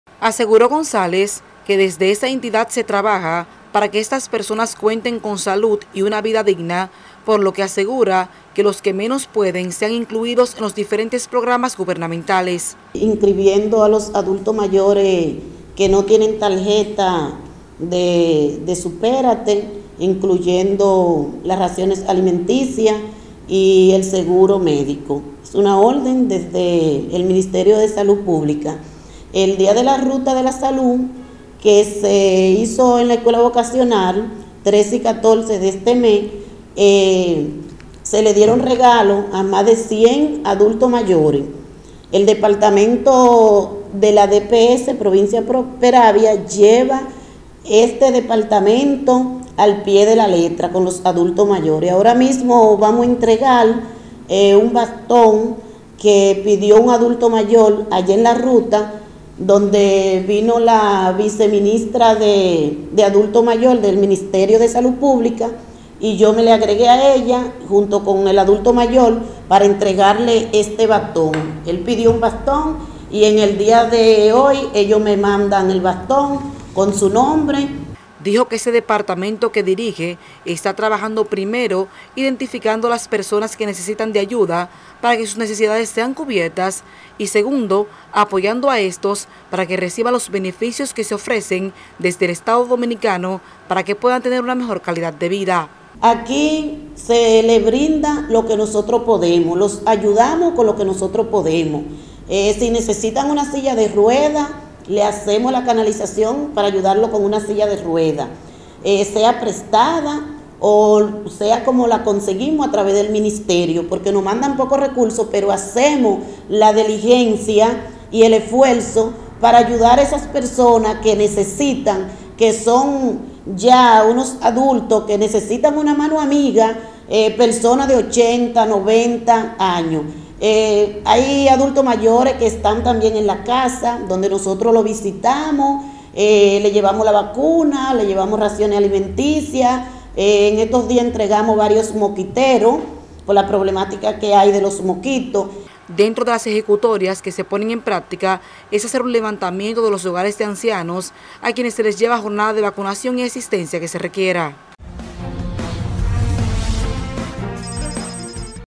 Departamento de adulto mayor de DPS Peravia habla sobre programas de ayuda
La encargada del departamento de adulto mayor de la Dirección Provincial de Salud Pública, licenciada Sandra González, afirmó hoy que desde esa dependencia se encuentran inscribiéndolos en los programas de ayuda del gobierno.